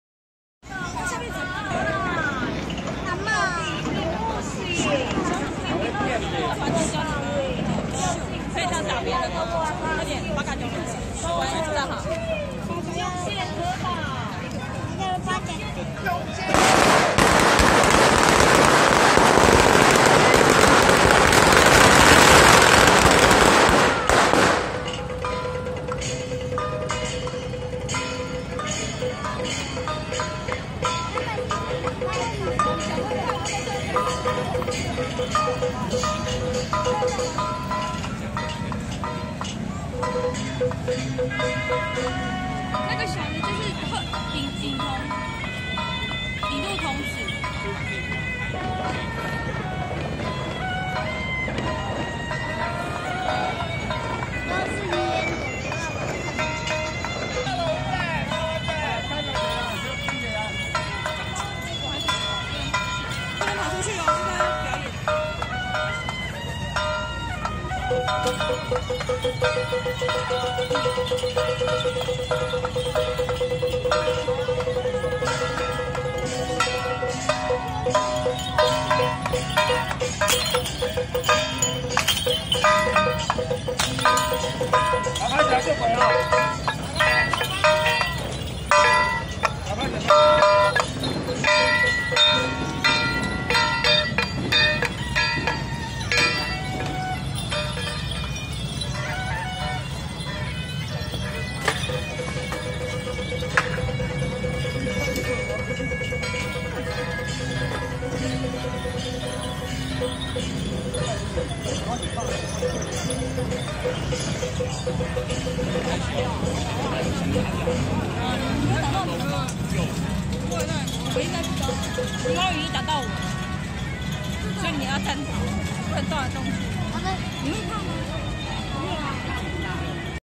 106.12/9艋舺青山宮靈安尊王聖誕日巡遶境【新北昱辰軒-八家將】
新北昱辰軒-八家將